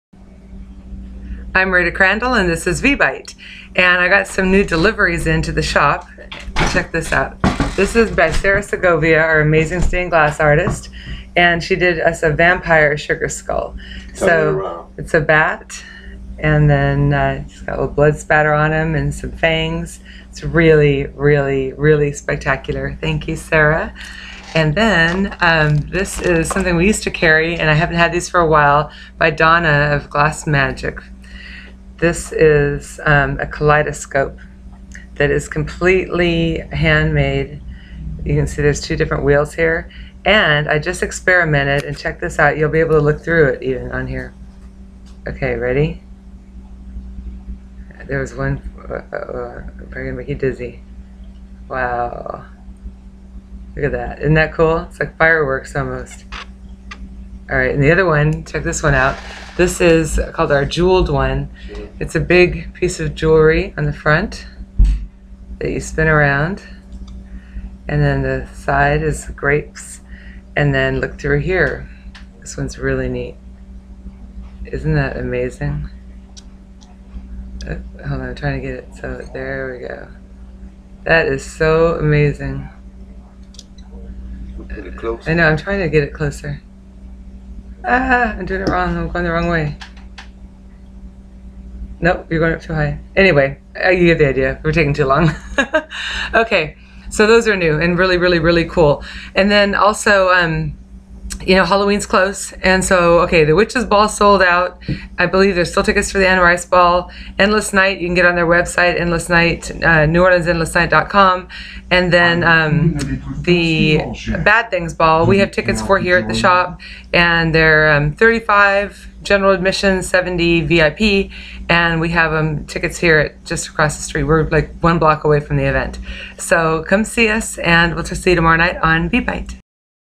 VBITE, October 18, 2014
Vampire Sugar Skull by Sarah Segovia
Kaleidoscopes by Dawna of Glass Magik
New Orleans Halloween Ball update